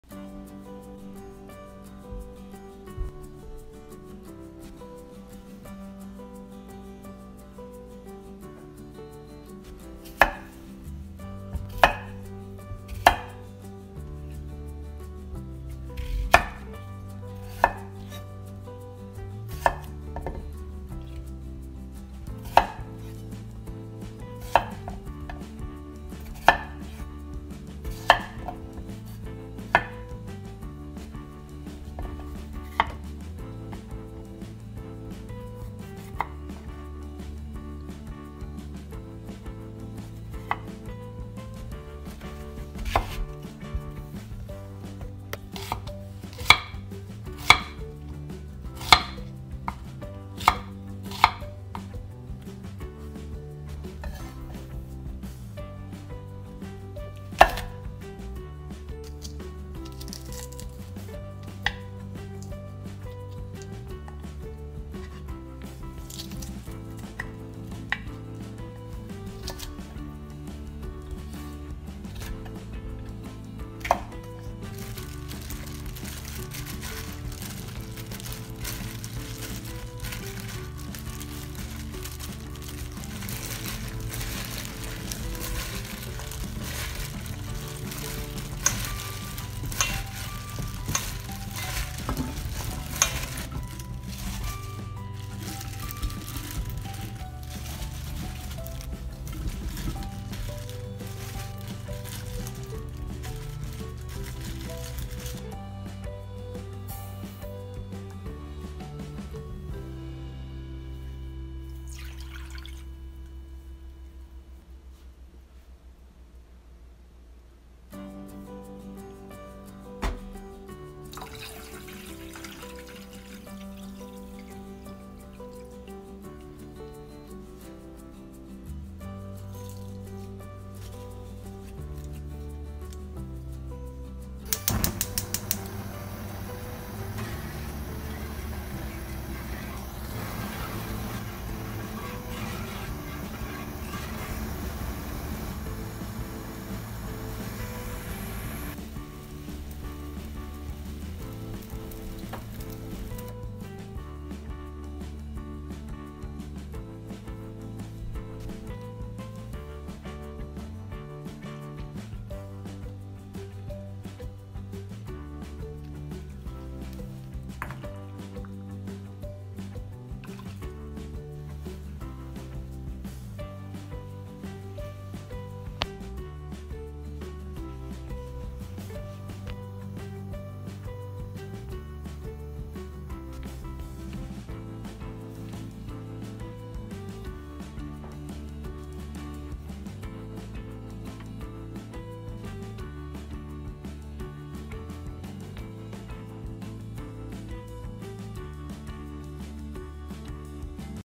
아삭아삭 맛있는 [오이피클] 만드는방법 / 만들기 간단하고 다음날 바로 먹을수 있는 오이피클 만들기
[재료]
오이6개, 양파큰거1개, 청양고추7개
물1L, 식초150ml, 설탕200ml,
소금3스푼, 통후추1스푼반,월계수잎2개